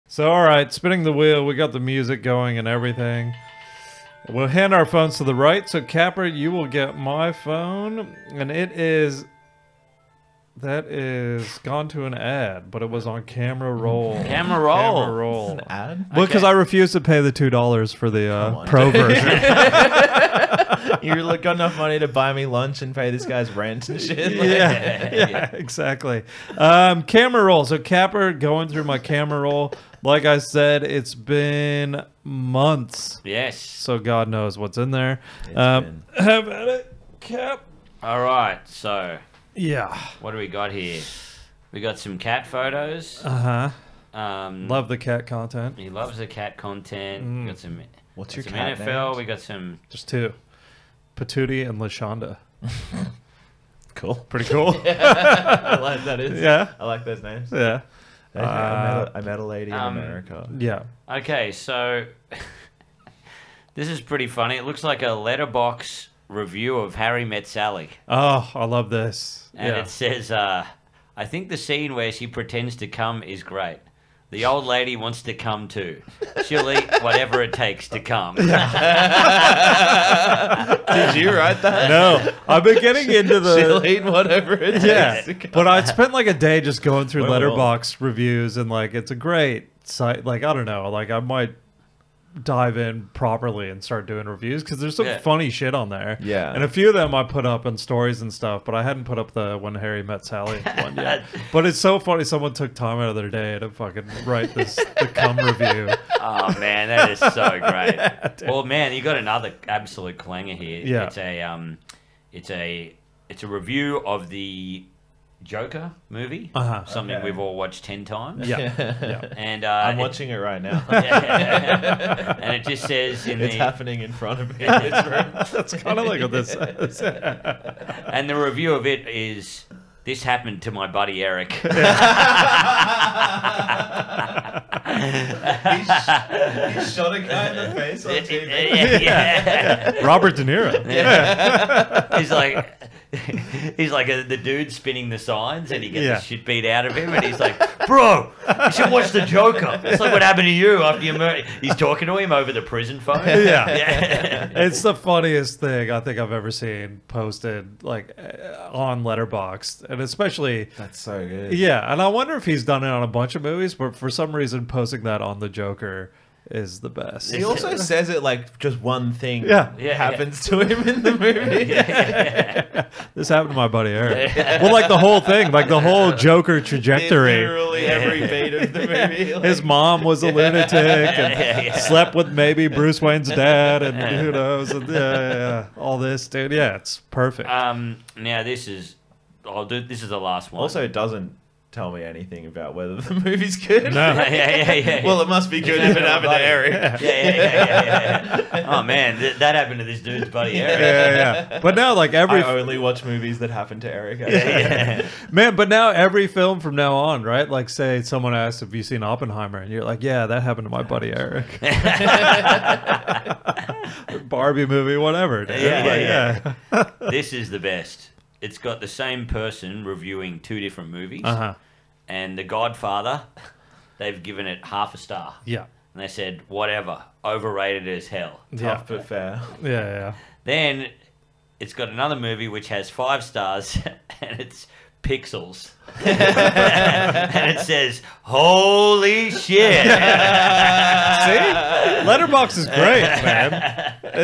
Funniest Letterboxd Reviews
Rohan Ganju joins us as we go through Mike's screenshots and find some funny Letterboxd reviews
Just $7 (AUD) for bonus eps and content.

Get tons of behind the scenes hacks and pranks and help keep this podcast going!